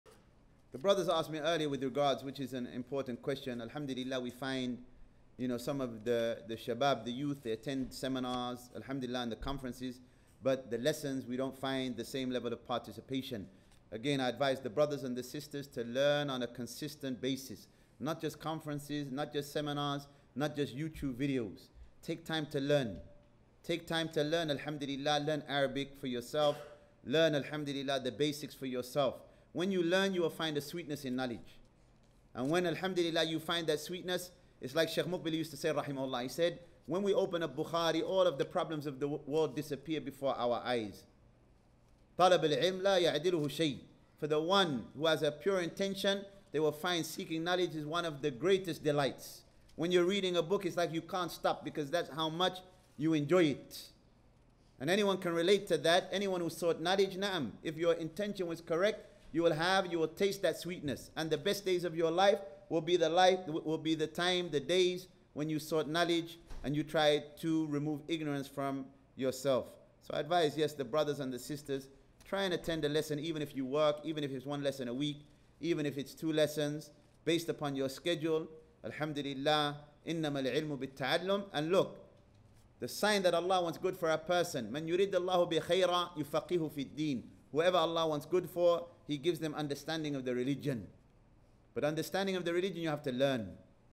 How Do We Encourage People to Attend Lessons and Graduate in Knowledge? - Ḥasan al-Ṣumālī
Shaykh Ḥasan answers a question on how to progress beyond attending conferences and one-off events and getting into the rhythm of weekly lessons.